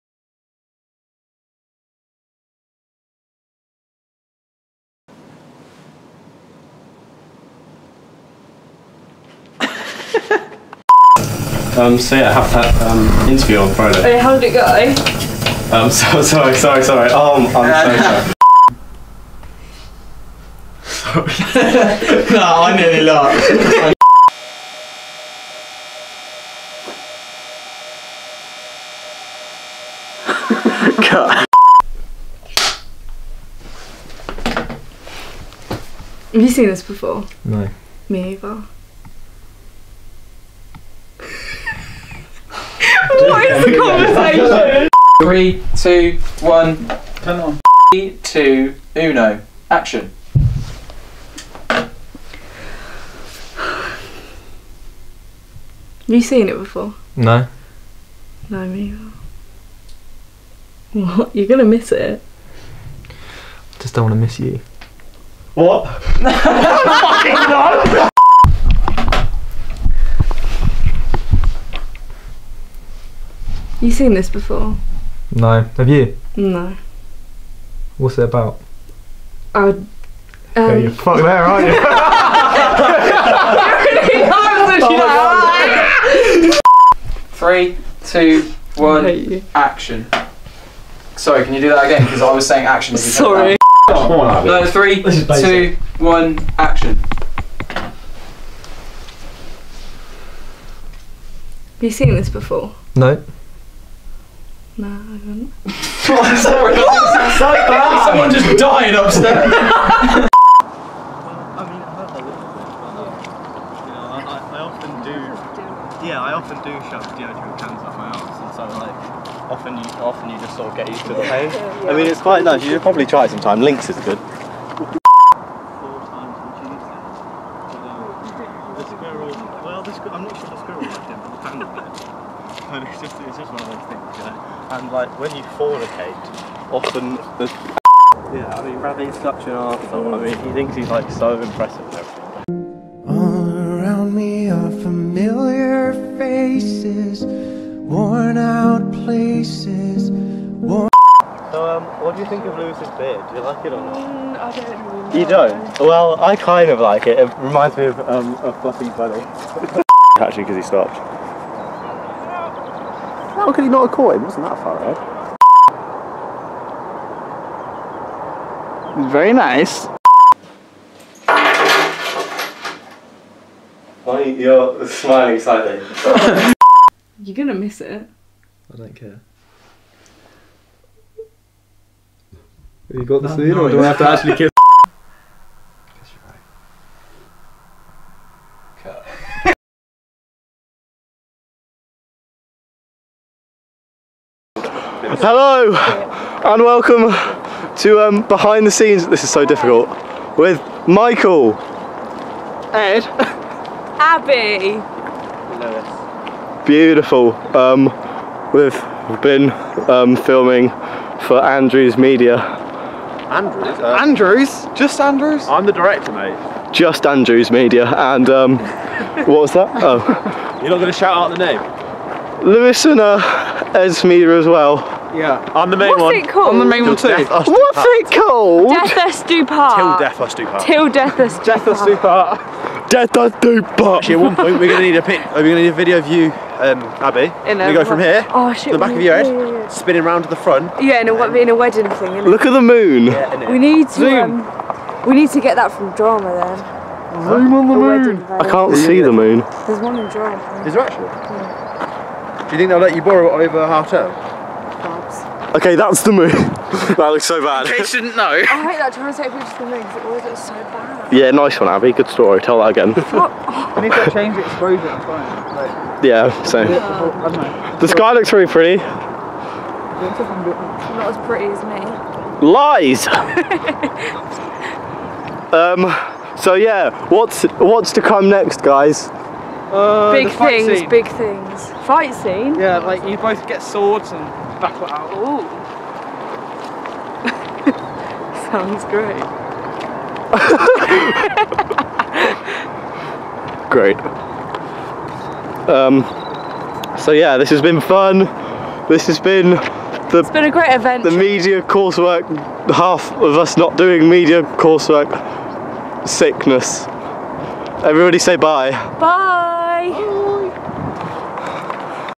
TDDUP - Blooper Reel
The blooper reel for our A2 Media trailer, 'Til Death Do Us Part'.

Thanks for the fun times lads :)